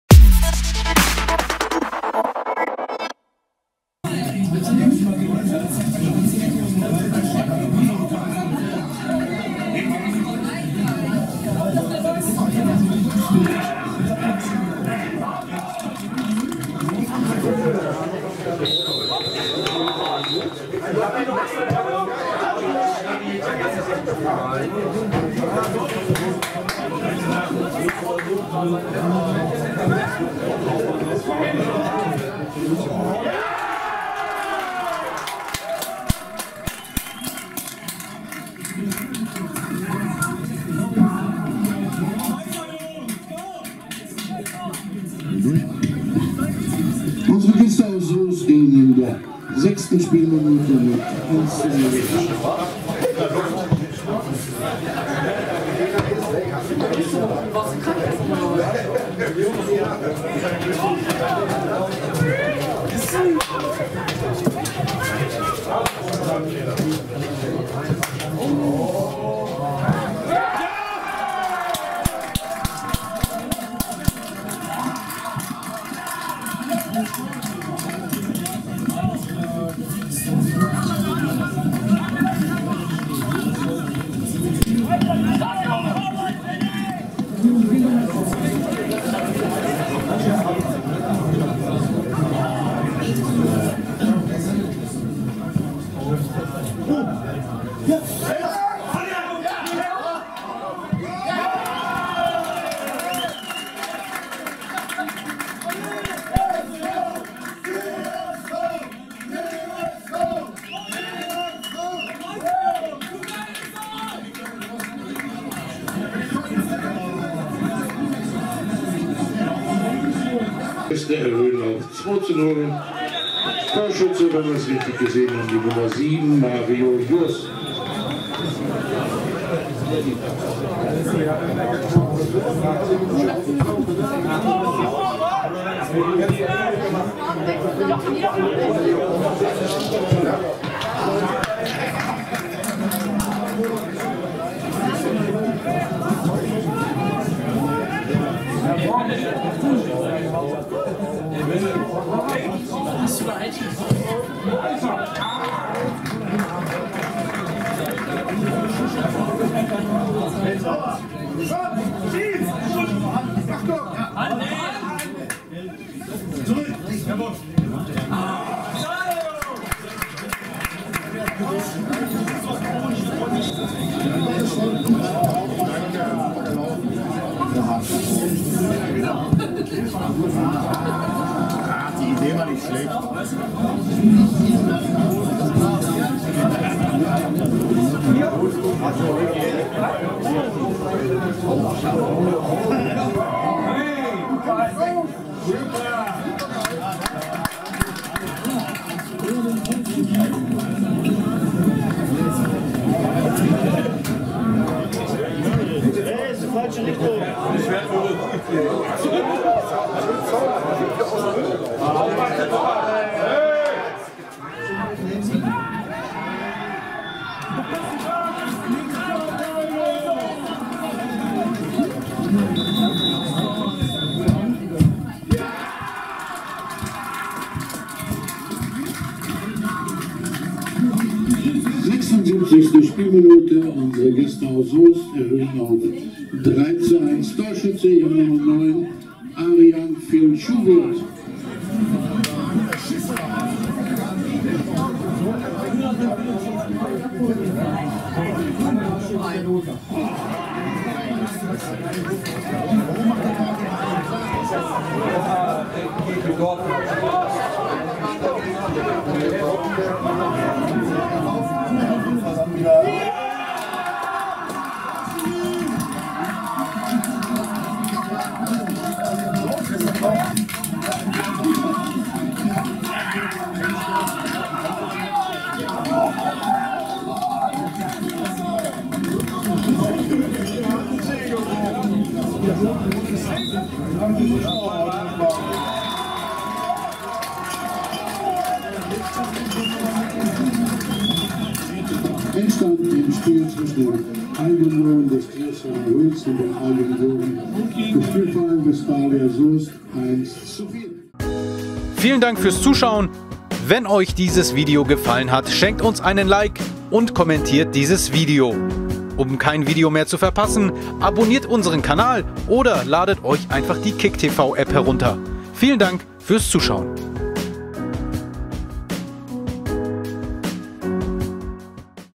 TSV Marl-Hüls U19 - SV Westfalia Soest U19 (13. Spieltag, Landesliga Staffel 2)
Soest erobert Tabellenspitze!

13.Spieltag (24.02.19)

Tore:
0:1 Tom Franke
0:2 Ricardo Puppe
1:2 Robin Slawinski
1:3 Ricardo Puppe
1:4 Mario Jurss

Aufstellung TSV Marl-Hüls U19:
1 Paul Mrosek-Graul, 2 Jan Philipp Schramm, 4 Jannes Terlau, 5 Maximilian Sellinat, 7 Robin Slawinski, 9 Arian Phil Schuwirth, 10 Semih Gülsoy, 13 Emre Gökkaya, 17 Bünyamin Oguz Celen, 18 Hendrik Christian Löbler, 23 Louis Jozek

Ersatzspieler:
3 Lukas Pfeiffer, 11 Lukas Tombrink, 15 Niklas Fink, 16 Sven Hahnenkamp, 21 Kacper Slupski, 24 Malte Fabisiak, 25 Moussa Sangare, 28 Maurice Rottenberg

Trainer:
Sebastian Nowak und Jörg Breski

Aufstellung Spielverein Westfalia Soest U19:
1 Lukas Kasparek, 2 Mic Degner, 3 Adrian Dinkel, 4 Philipp Ratz, 5 Mohamed Lamine Kourouma, 6 Luke Neitzner, 7 Mario Jurss, 8 Joshua Becker, 9 Ricardo Puppe, 10 Tom Franke, 11 Niclas Dohnisch

Ersatzspieler:
12 Maik Razlaf, 13 Kjell Vierus, 14 Alexandre Köthschneider, 15 Marius Andreas Radon, 16 Amadeus Chronz, 22 Tugay Özdemir

Trainer:
Ibou Mbaye